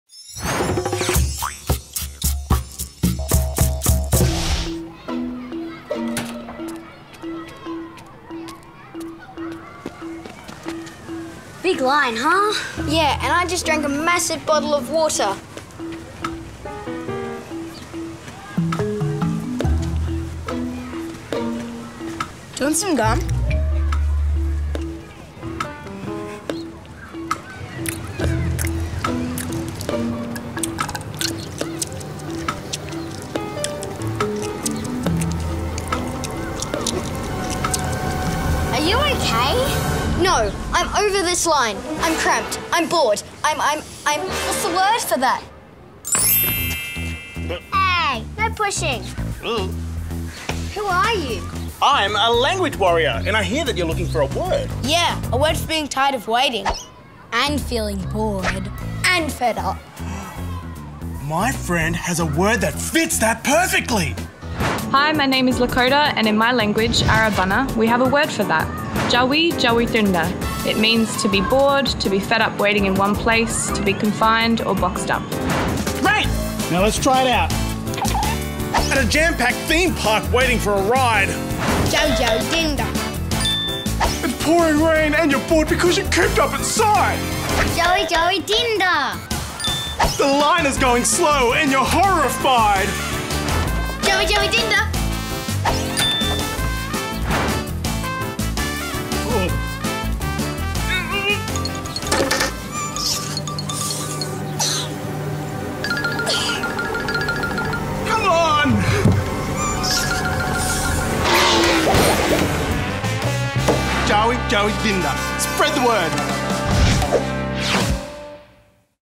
Spread The Word: Tyawi-tyawithirnda
Tyawi-tyawithirnda: You’re waiting in a long line. You’re annoyed and bored, and you just had enough. Familiar? For sure. Do you have a word for that? The South Australian Arabana community does! Tyawi-tyawithirnda: it’s a bit long, but not as long as the line for the toilet in this episode!